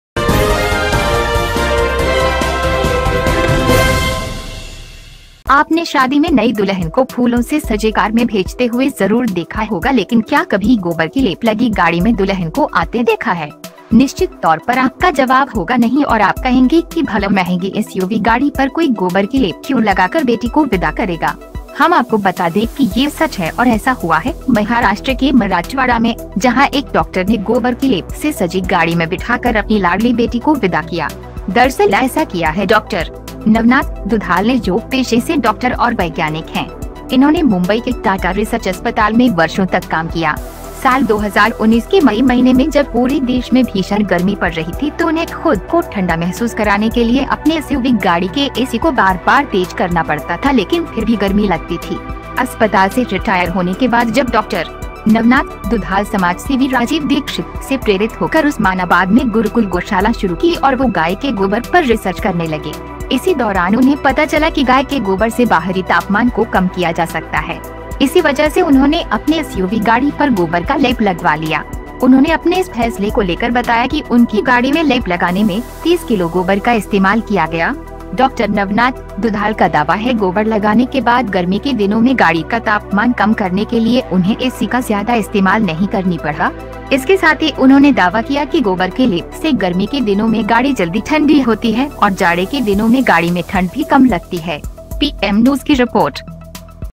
डॉक्टर ने गोबर से लिपी SUV से किया बेटी को विदा, वजह जानकर करेंगे वाह-वाह
Doctor leaves daughter with cow dung from SUV, will know why

You must have seen the new bride sending her to a car adorned with flowers at the wedding, but have you ever seen a bride coming in a car covered with cow dung? Surely your answer will be no, and you will say that why would someone bid farewell to a daughter by applying cow dung on an expensive SUV? Let us tell you that this is true and it has happened in Marathwada, Maharashtra, where a doctor, in a car decorated with cow dung paste, sent his beloved daughter off.
Actually, Dr. Navnath Dudhal, who is a doctor and scientist by profession, has done this. He worked at the Tata Research Hospital in Mumbai for years. In the month of May 2019, when the entire country was experiencing intense heat, they had to repeatedly accelerate the AC of their SUV vehicle to make themselves feel cold but still it was hot.
After retiring from the hospital, when Dr. Navnath Dudhal started a Gurukul Gaushala in Osmanabad, inspired by social activist Rajeev Dixit and he started doing research on cow dung. During this time, he came to know that the cow dung can reduce the external temperature. For this reason, he got cow dung coated on his SUV.
He told about his decision that 30 kg of dung was used to apply paste in his car. Dr. Navnath Dudhal claims that after applying cow dung, he did not have to use AC much to reduce the temperature of the car during the summer. Along with this, he claimed that due to the application of cow dung, the car cools quickly during the summer days and the cold also reduces in the winter.